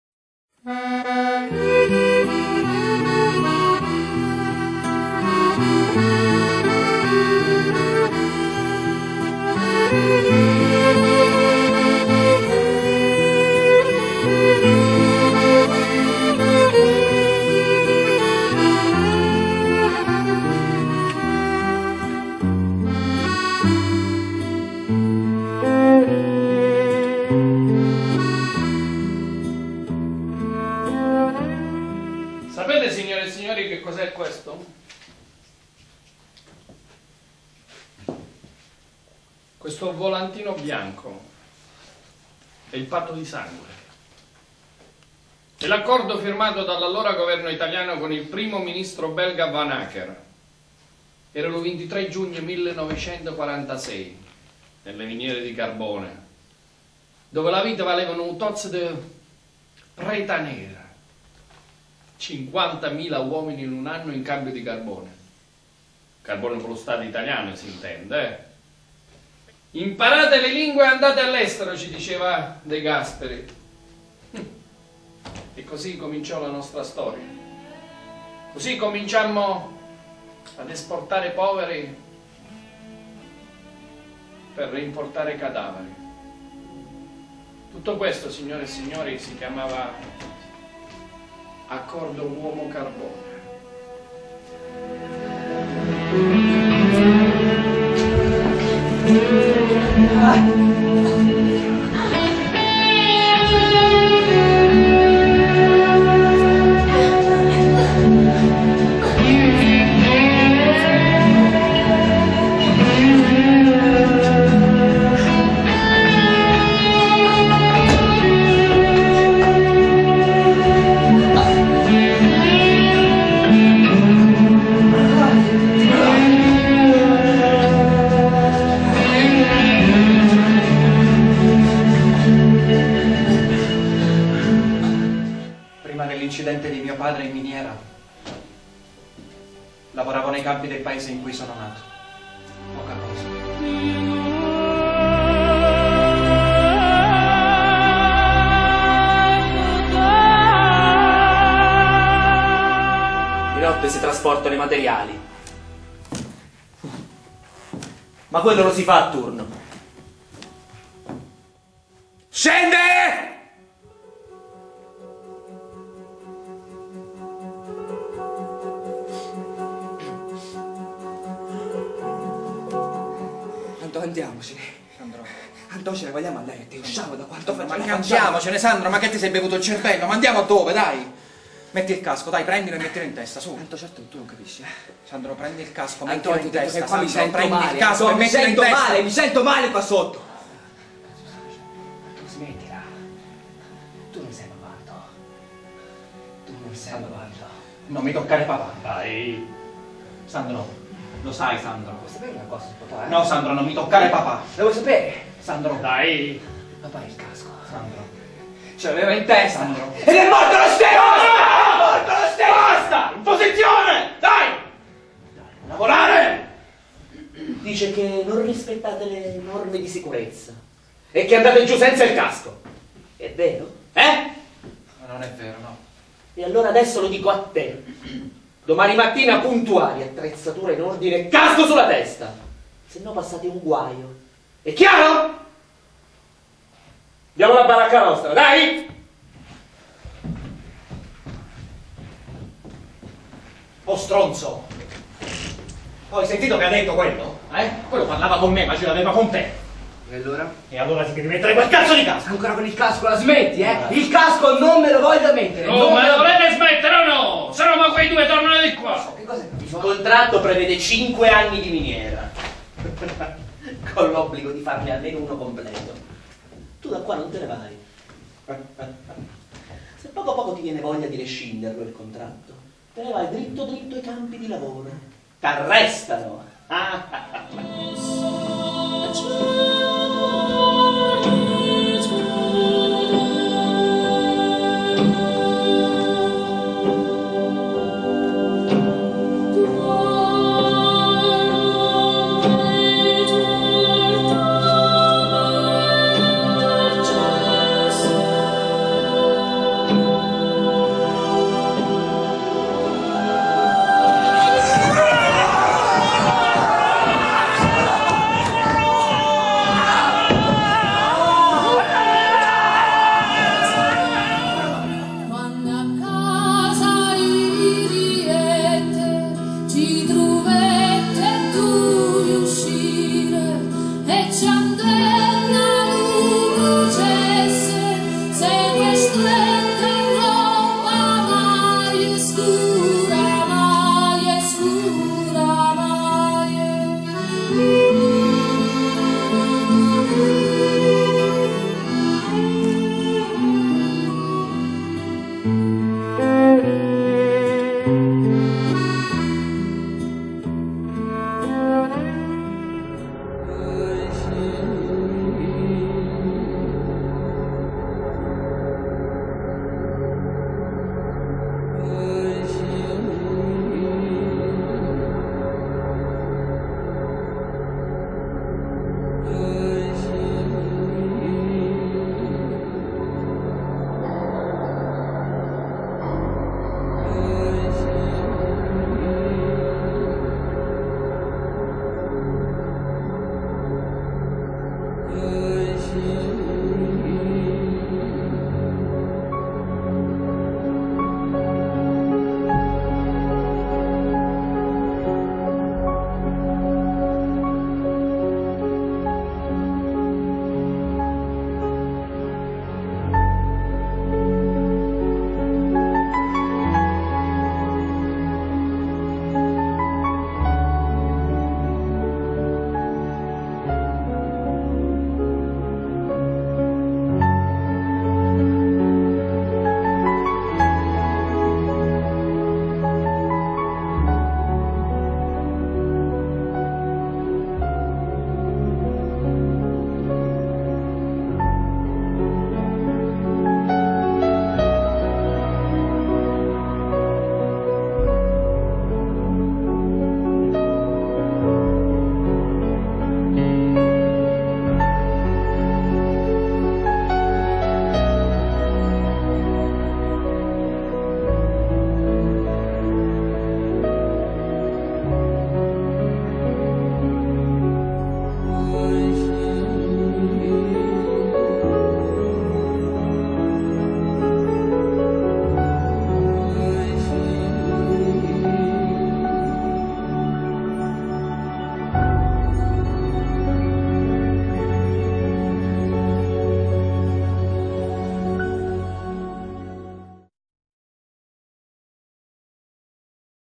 l'Uomo-Carbone (Trailer)
Regia di Federica Vicino.
Testo di Michele Di Mauro e Marco Finucci
Interpreti: Michele Di Mauro, Alessandro D'Incecco e Pierfrancesco Leone.
Altri Interpreti: Lina Bartolozzi, Rita De Bonis, Nicky De Chiara, Massimo Leone, Rossella Remigio, Emiliano Scenna
e con Pino Cifaratti, Barbara De Felice e Monica Verì.
Video a cura di Miriam Palmarella

Il Teatro Sociale di Pescara propone lo spettacolo intitolato "L'uomo - carbone", una performance teatrale dedicata alla tragedia di Marcinelle.
L'8 agosto del 1956, a Marcinelle, nei pressi di Charleroi, in Belgio, un incidente devasta la miniera detta Bois du Cazier, provocando una strage: 262 dei 274 minatori presenti in quel momento all'interno della miniera perdono la vita. La maggior parte di loro è di origine abruzzese.

La compagnia del Teatro Sociale di Pescara, da tempo impegnata sulle tematiche del rapporto fra letteratura e lavoro, arte e lavoro, teatro e lavoro, ha
voluto affrontare anche la vicenda dei minatori del Belgio, con uno spettacolo che ripropone fedelmente dati ed elementi tratti dal contesto storico-politico dell'epoca, ma è  imperniata su una storia di fantasia, dal taglio "neorealista", molto intima.